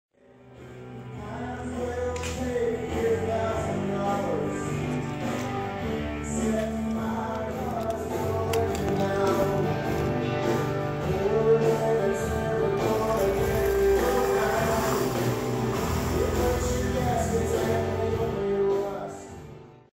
Prototyping some mods for a Nerdy Derby track
Testing some construction ideas for a Nerdy Derby track, including alignment guides where the sections bolt together and bolts to clamp the layers together.  Probably need to work on the support system a little though...not sure a forklift is cost effective.